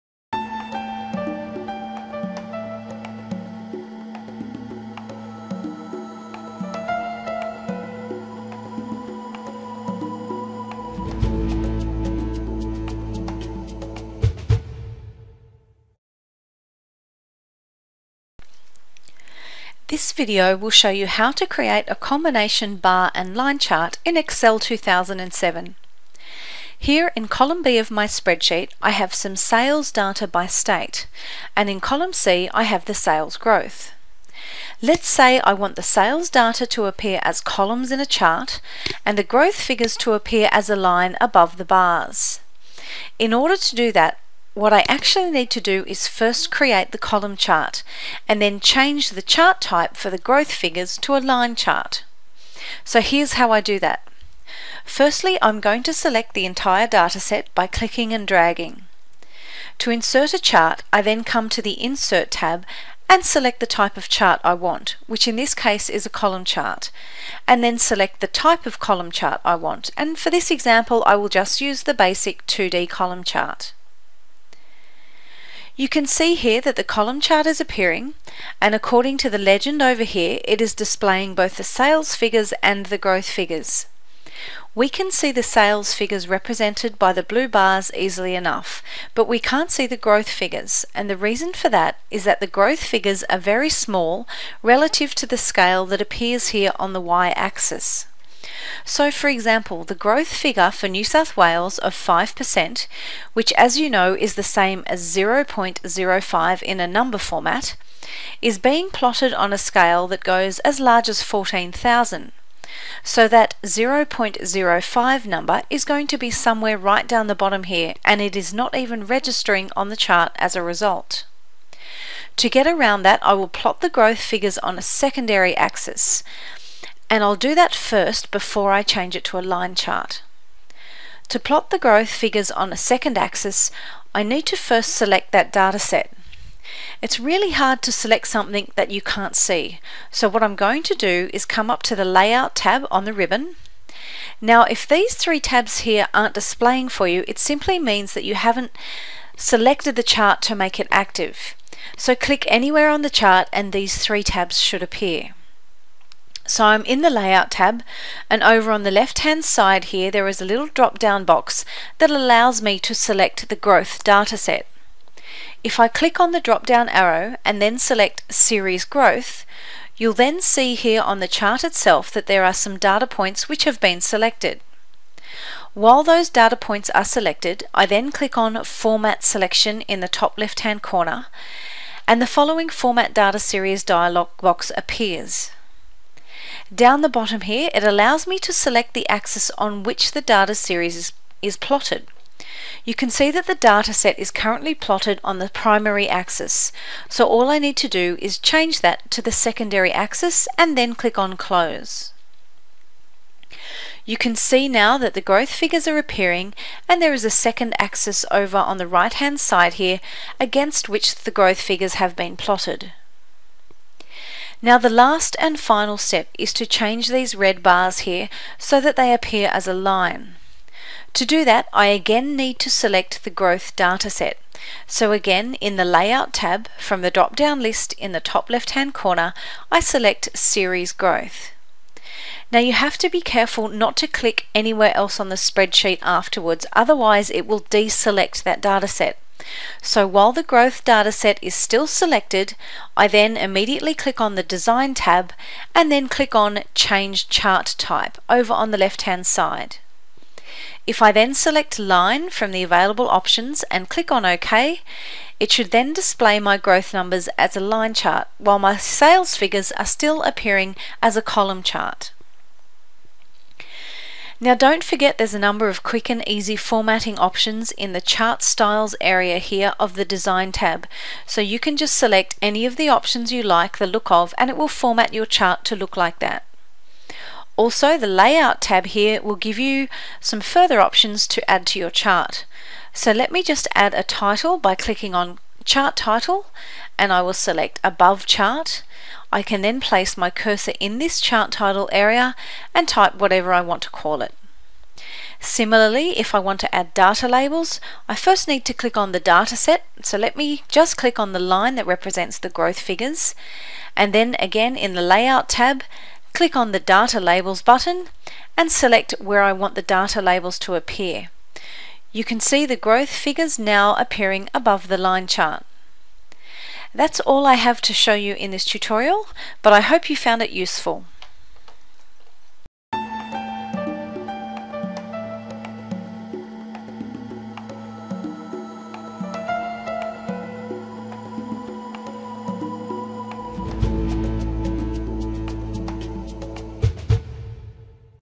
How to Create a Combination Bar & Line Chart in Excel 2007
This video will show you how to create a combination column or bar chart coupled with a line chart in Excel 2007.  This type of chart was previously found under "Custom Chart\Line - Column" in Excel 2003.